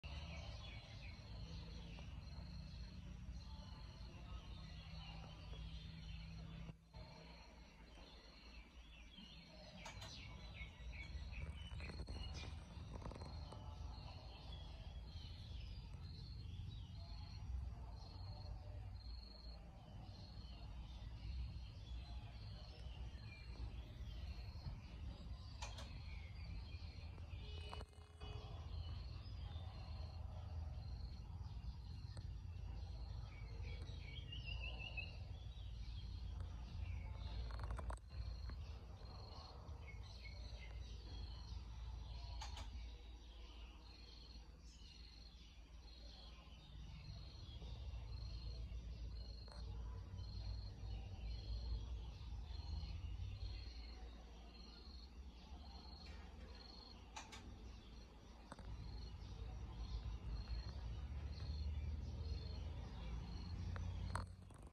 Caterpillars eating bonsai || khunman
"The caterpillars ate all the bonsai trees I planted in my backyard."